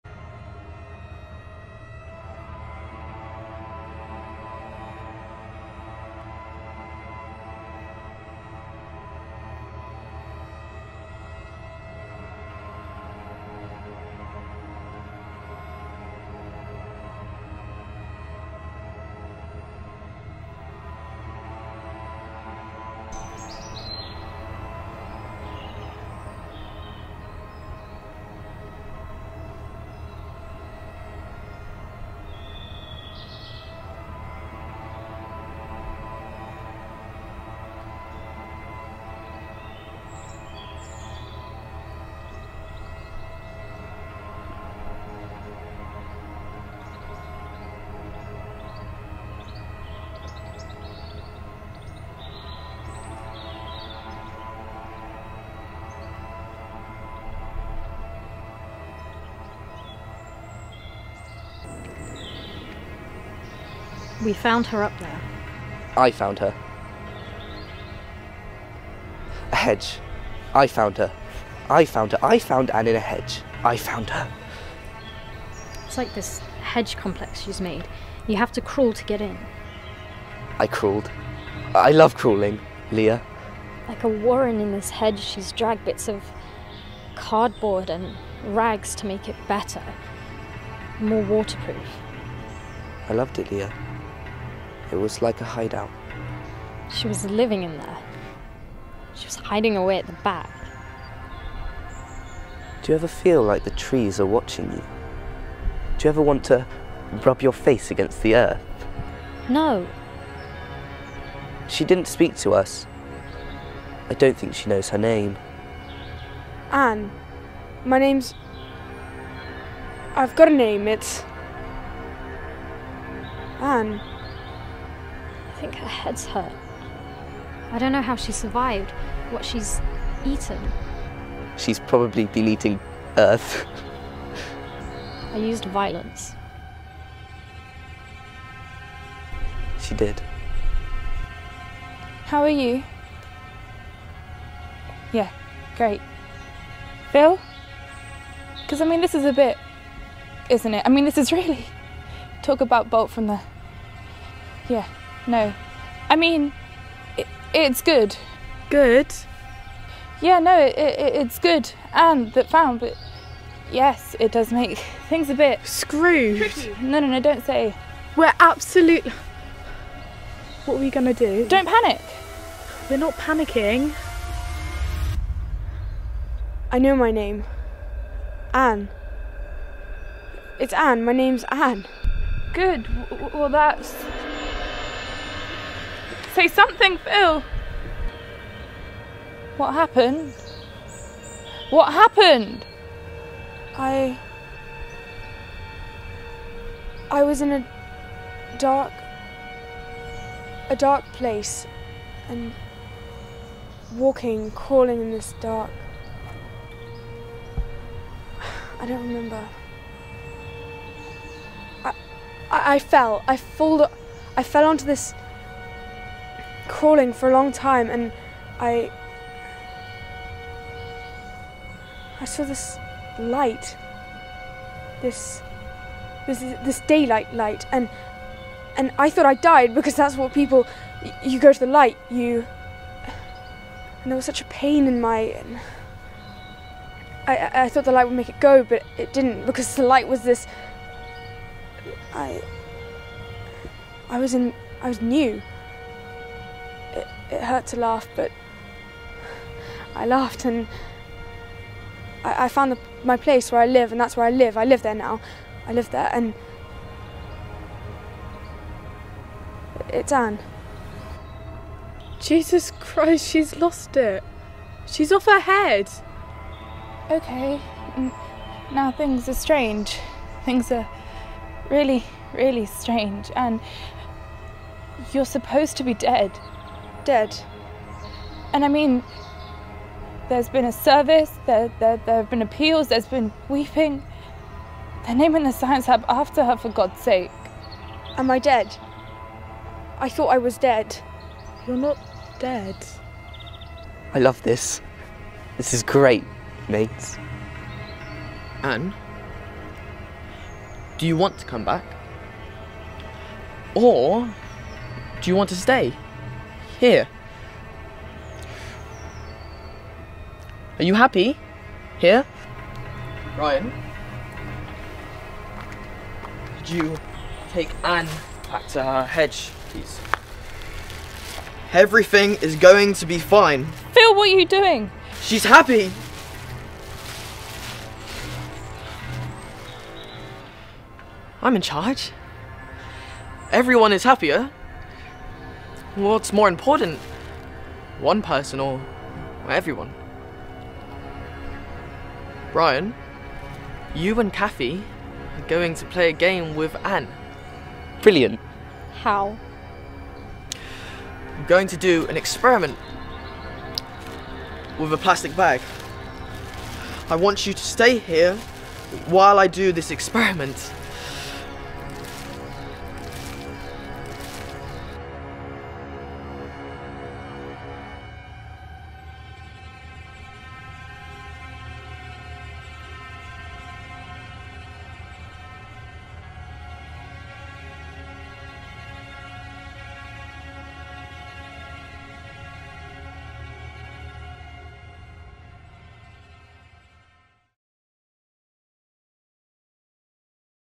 An excerpt from DNA by Dennis Kelly
Performed by ex-APA Talent actors. Directed by Marcus D'Amico. Filmed by Steele & Coe Films.